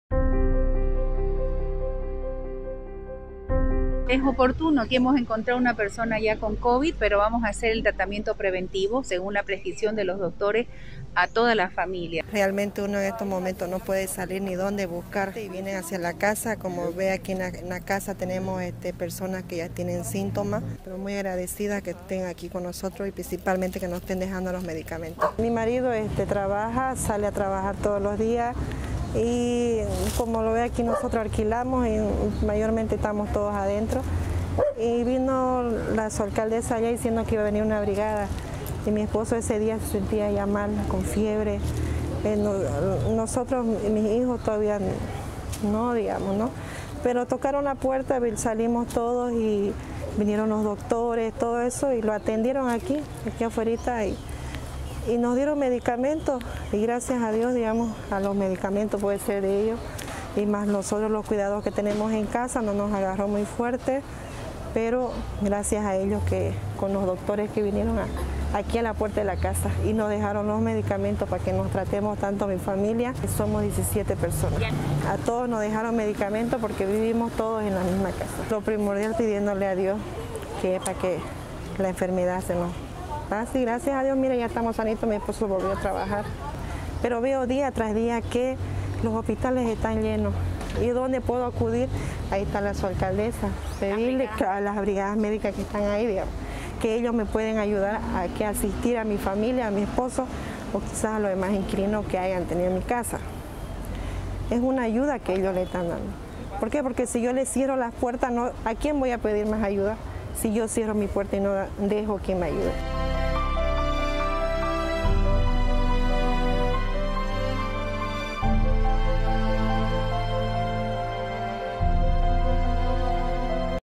Familia que padeció con coronavirus, mejora su salud por la atención de brigadistas
El Gobierno Municipal de Santa Cruz de la Sierra con su  “Plan Todos Por La Vida”, llega de manera oportuna a los vecinos que padecen coronavirus. Es el caso de la familia Leigue que vive en la Pampa de la isla. Casi una decena de personas padecieron el mal. El 8 de julio las brigadas médicas dieron un tratamiento y dotaron de medicinas a esta familia donde ahora todos gozan de buena salud. La alcaldesa interina, Angélica Sosa, y los médicos brigadistas, visitaron a la familia que expresó su agradecimiento por el accionar de la comuna.